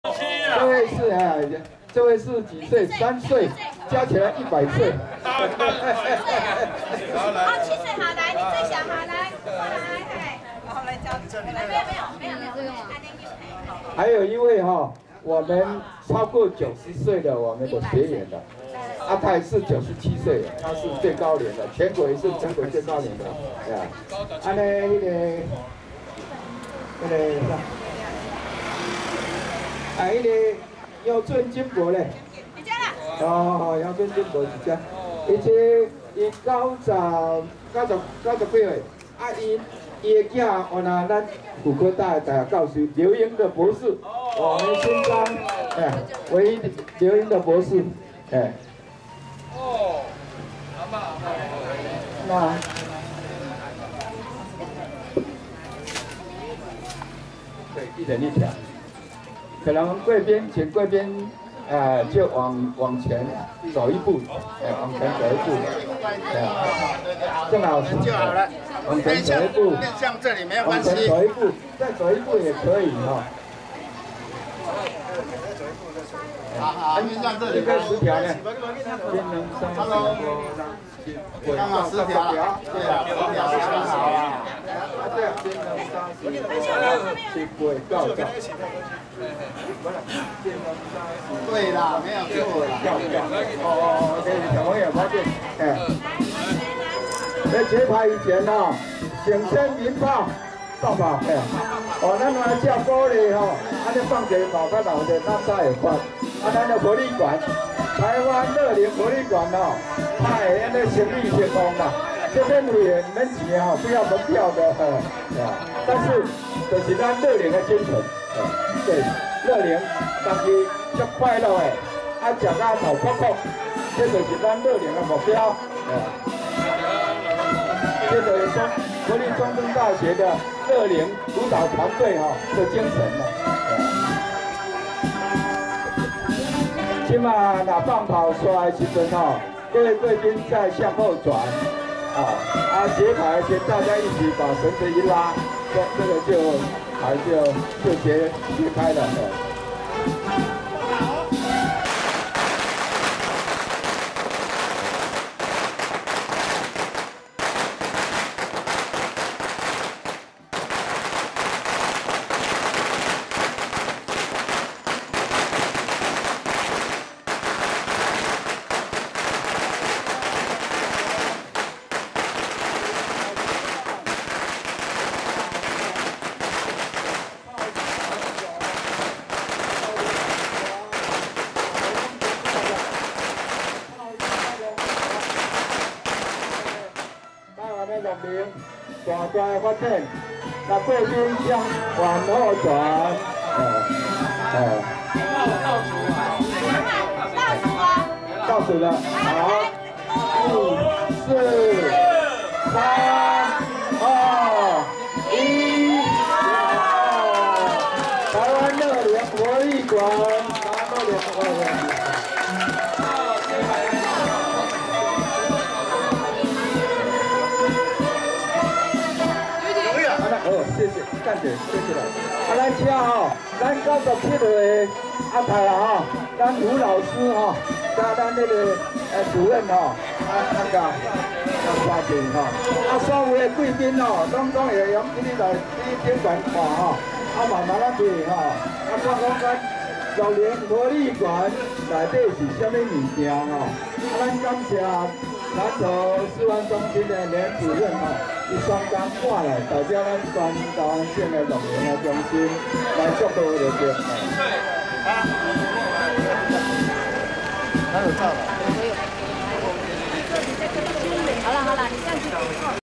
揭牌-台灣樂齡活力館開幕-6-雲林縣土庫鎮第一樂齡
全國第一座台灣樂齡活力館於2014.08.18.在新庄社區活動中心二樓揭牌開幕，新庄國樂社、馬光樂齡學員、信義育幼院學童，地方長官、雲林縣土庫鎮第一樂齡學習中心學員一起參加，除唱歌跳舞慶祝外，也一起體驗各項活動設施，大家都很快樂。